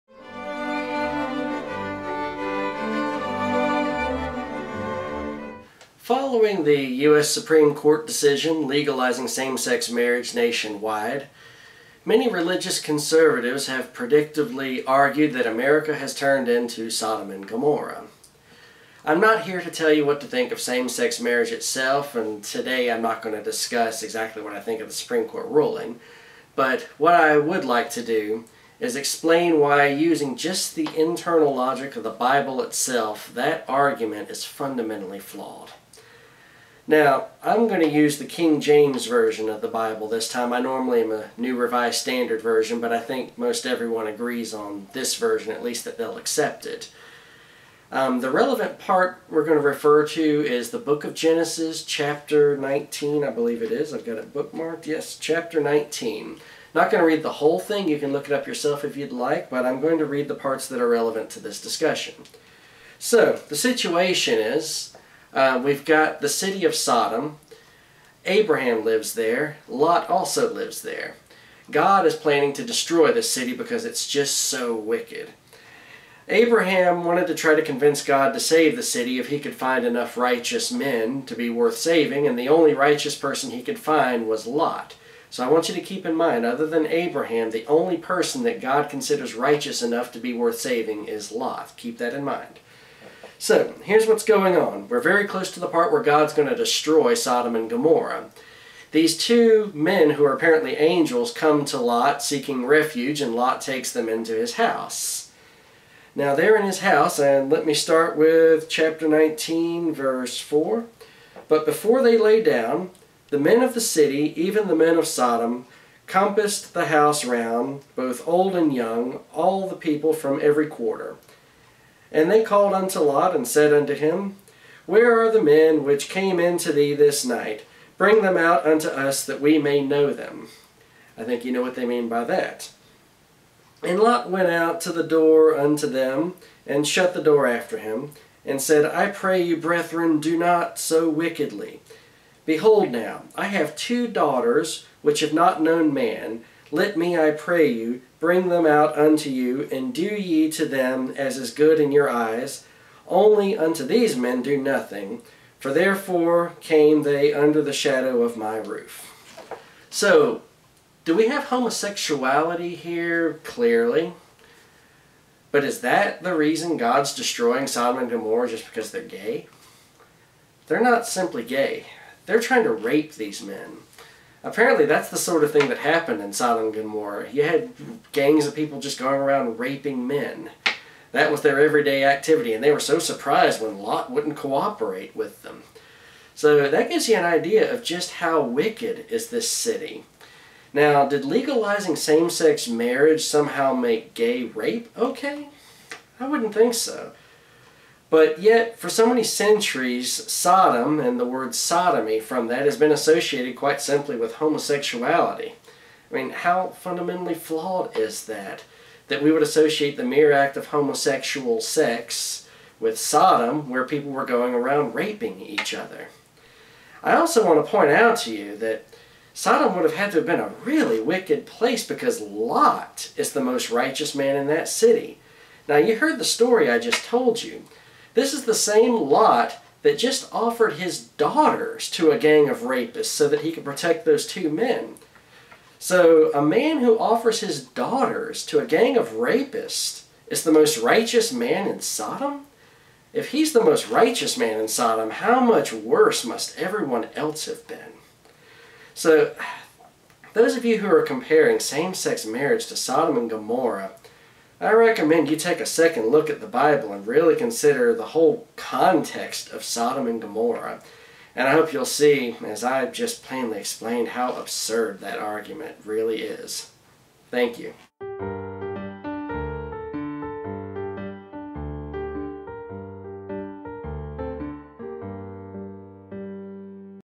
Gay Marriage - Is the US now Sodom and Gomorrah?
Professor Wag will debunk the claim that same-sex marriage has turned the US into the new Sodom and Gomorrah, as described in the Book of Genesis.

On another note, what this gay Christian man is doing for the Kleins (that cake baking couple that refused to make a wedding cake for a gay wedding and has been fined into bankruptcy) is commendable: